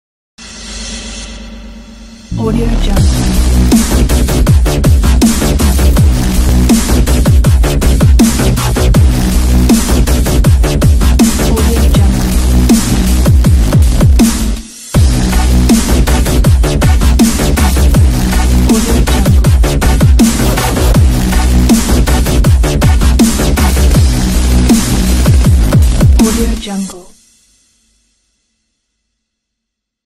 Production Show Reel | After Effects Project Files - Videohive template
Welcome to my After Effects project – Modern Production Demo Reel The project is perfect for your Demo Reels, Slideshows, Showrels as well as many other things… Use Photo and Video files,change colors and get a great, powerful, modern Showreel. The project is made in trending dynamic style and provide you with the perfect design your photos or videos! Changing text , drop your media, audio and hit render. Fast rendering project. Audio & Video Preview purpose Only.Not included Audio